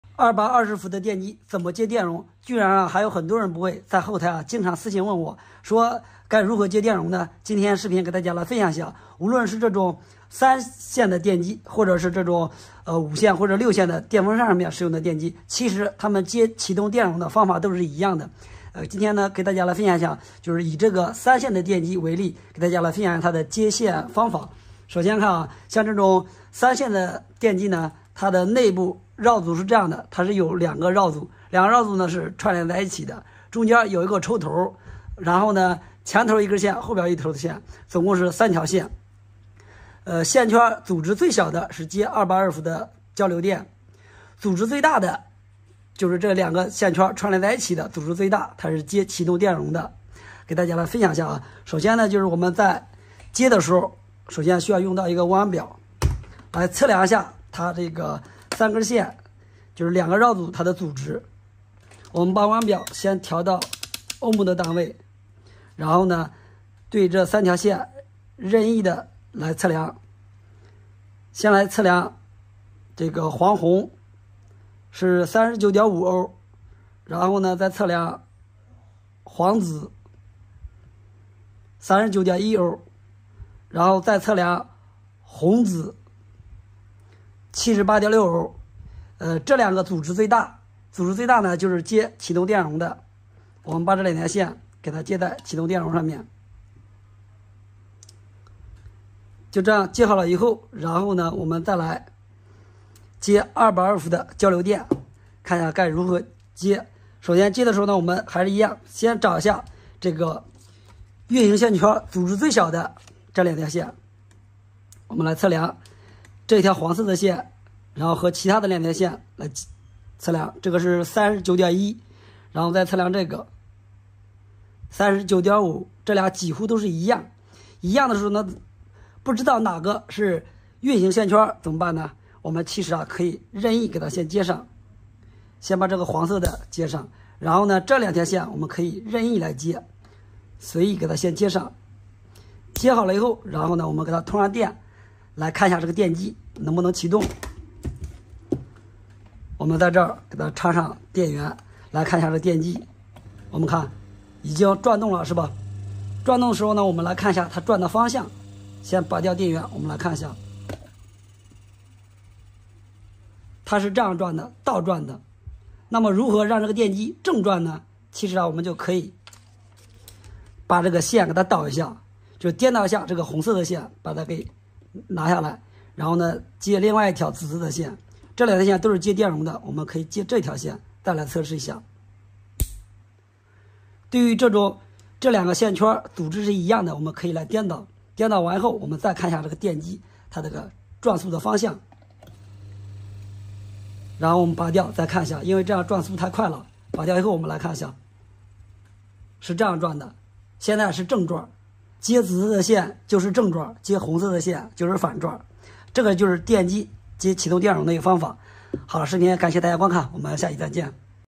220伏电机怎么接电容？怎么接电源线？方法简单一看就会
我们的视频是为了为观众提供有价值的内容，绝对没有任何违规行为。 非专业人员请勿直接操作！
哈喽，大家好
220伏电机怎么接电容？怎么接电源线？方法简单一看就会